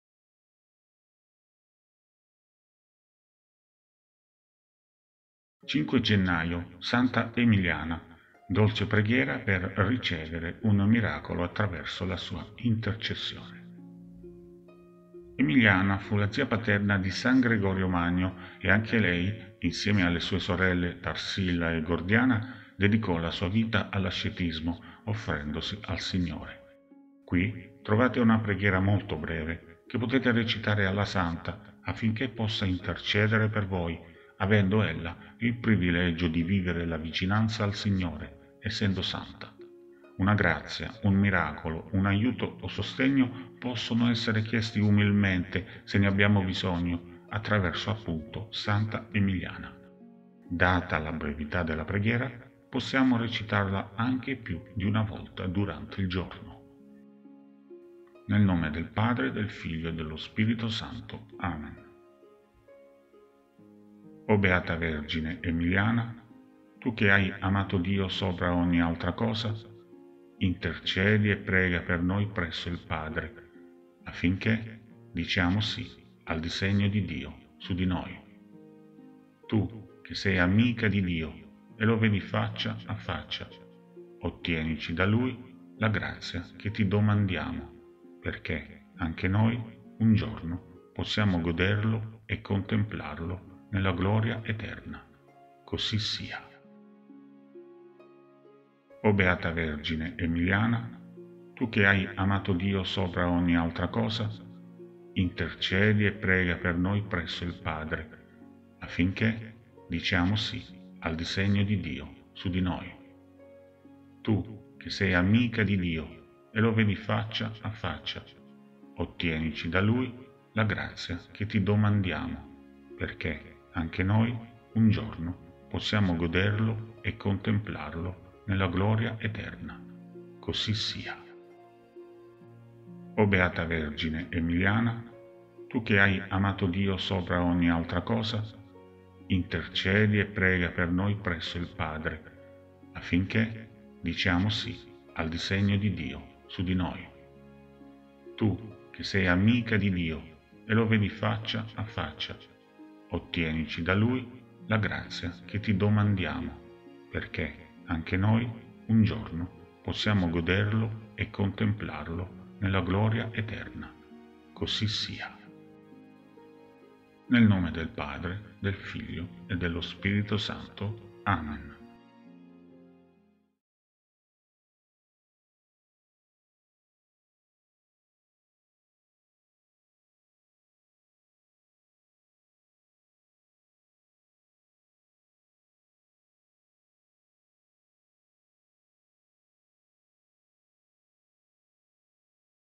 5 Gennaio Santa Emiliana | Dolce Preghiera per Ricevere un Miracolo Attraverso la sua Intercessione
5 Gennaio Santa Emiliana | Dolce Preghiera per Ricevere un Miracolo Attraverso la sua Intercessione